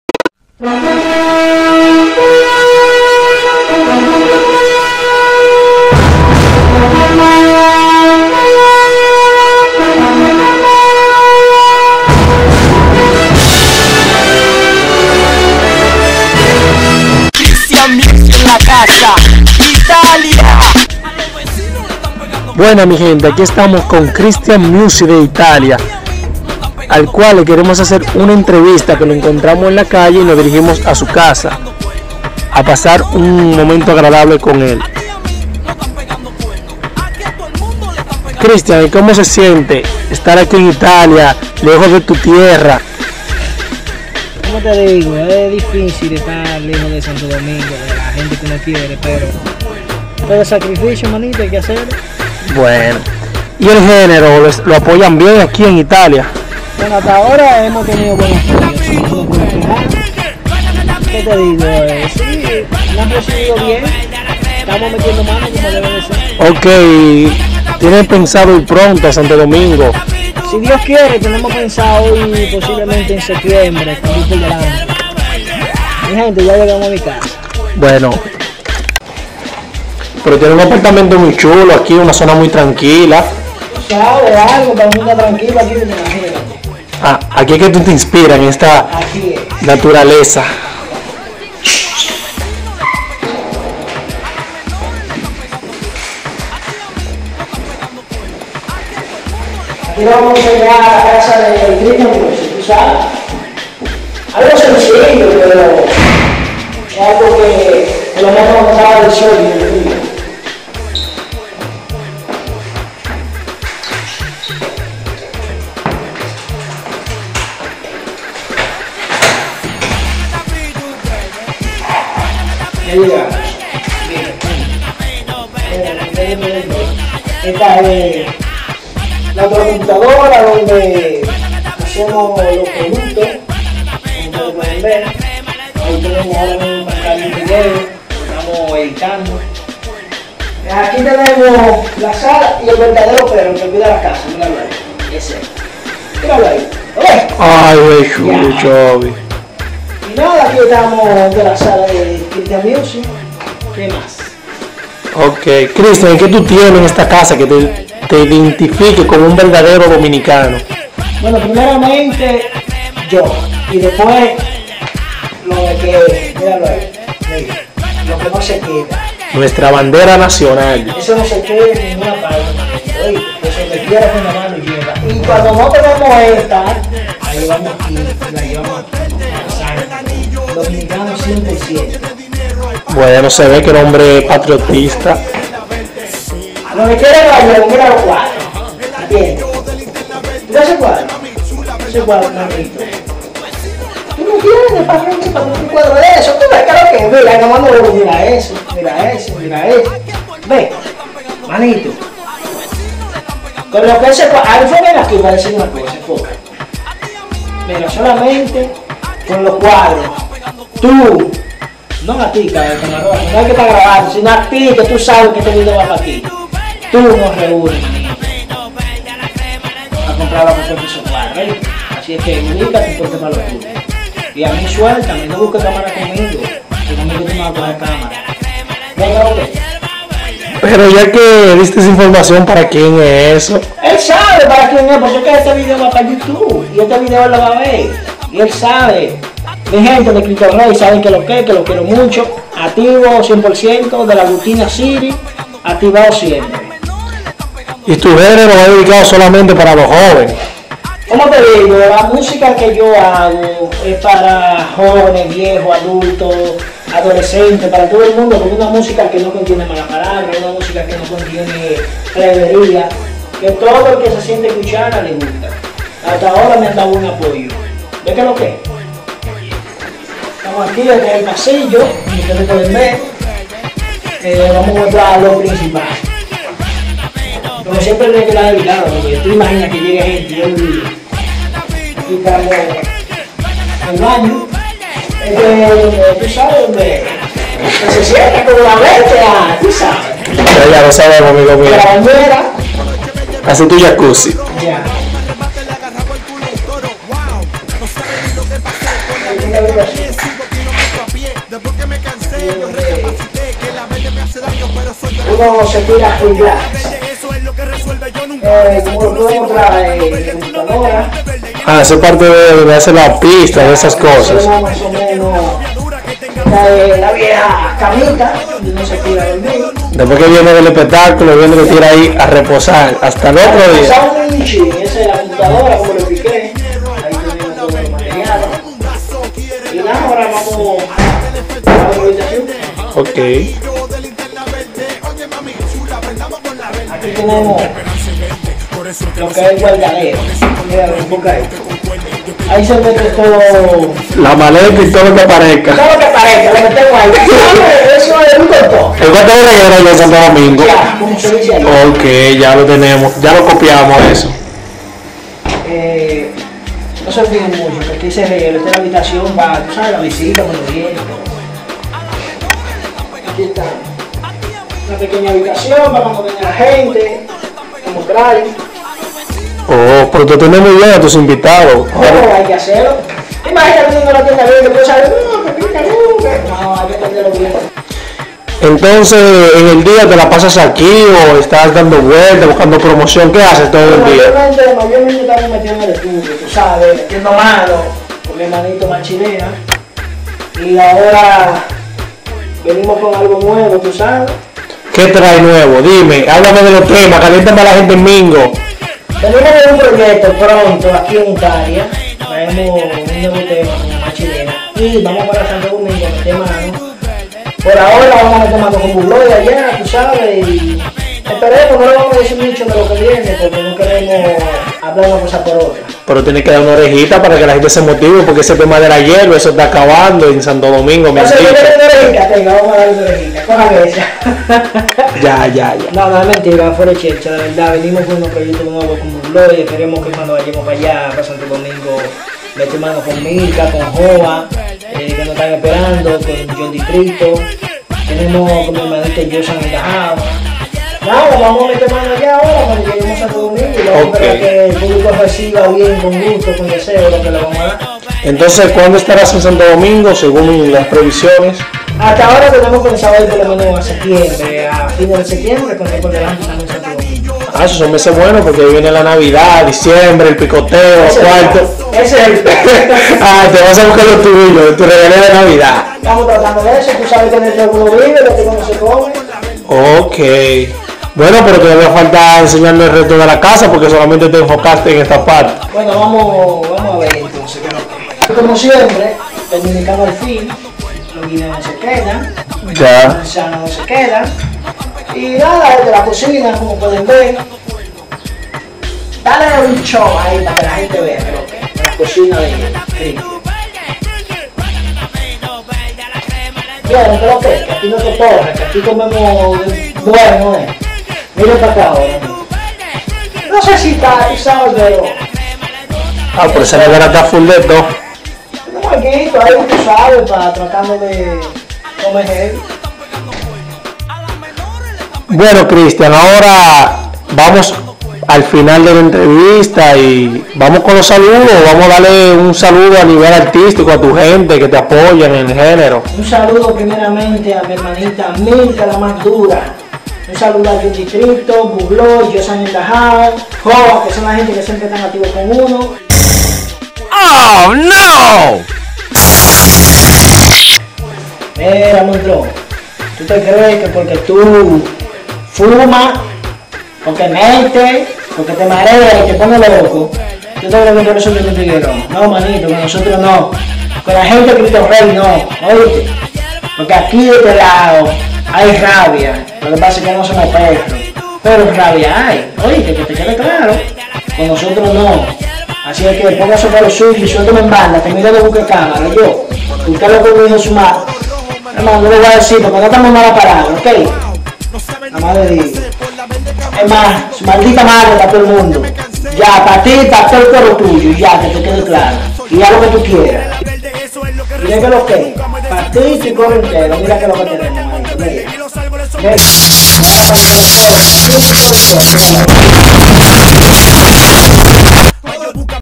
una tarde en la casa de christian music en italia
esto fue una tarde en la casa de christian music de italia